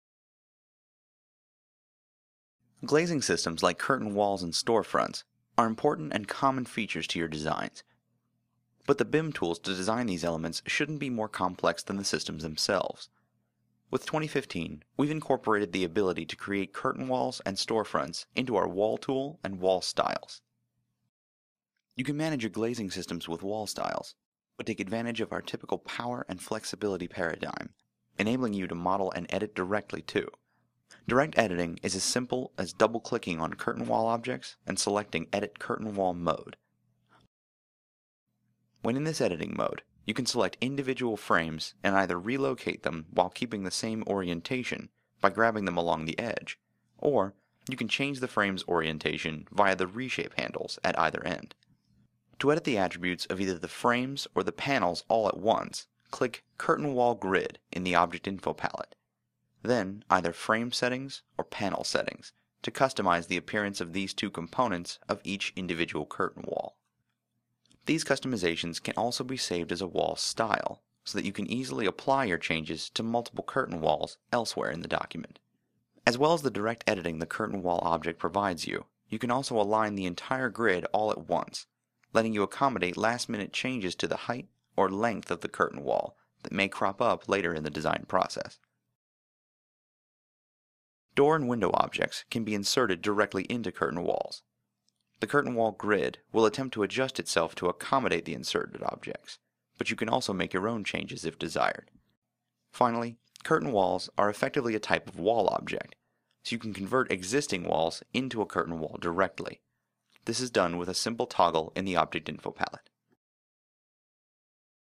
Curtain Walls
Glazing systems like curtain walls and storefronts are important features in your designs, but the BIM tools to design them shouldn’t be more complex than the systems themselves. With 2015, you can create curtain walls and storefronts with our Wall tool.